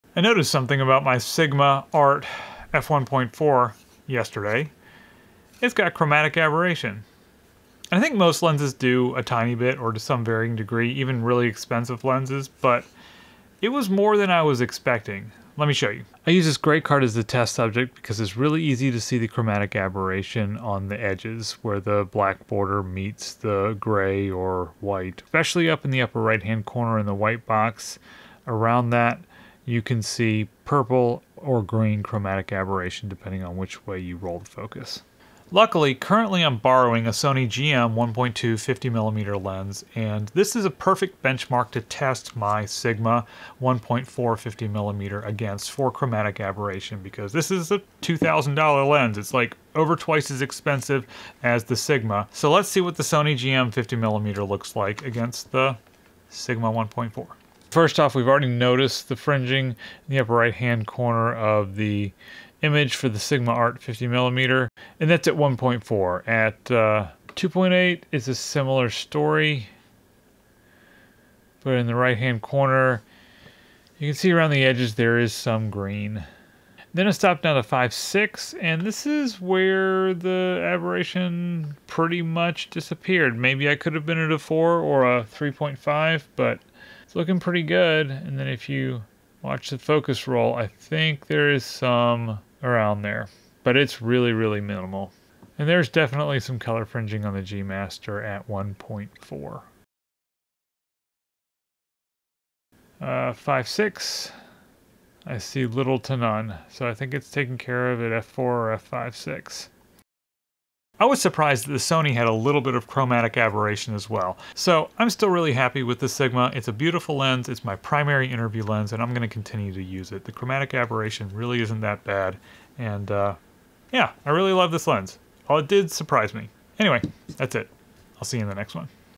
Sigma Art 50mm f1.4 vs Sony G Master 50mm f1.2 | Chromatic Aberration Test
Sigma Art 50mm f1.4 vs Sony G Master 50mm f1.2 | Chromatic Abberation

I recently noticed that my Sigma Art 50mm has chromatic aberration. This test is to see if the aberration is better or worse than the Sony G Master 50mm.